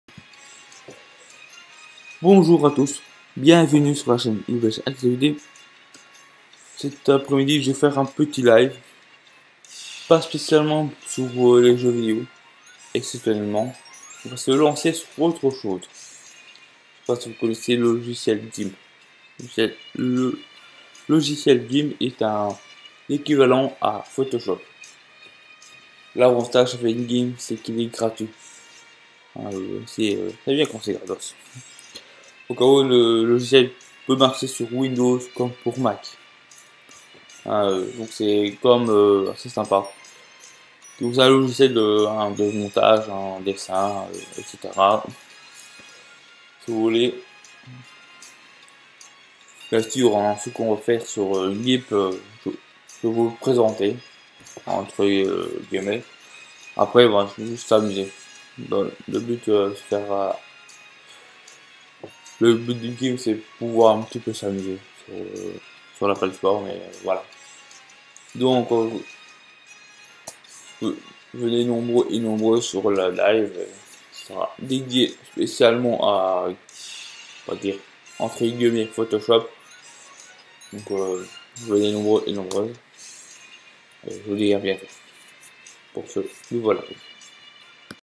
(Annonce) Je lance un live spécial cet-après midi
Bonjour a tous bienvenue sur la Chaîne Yubelge Activity
Je serais en live cet-après midi dédiée spécialement
à l'univers du montage ou du dessin sur logiciel sur Gimp
Gimp est un logiciel de montage de photo comme photoshop  a par que Gimp est gratuit
disponible pour Windows et Mac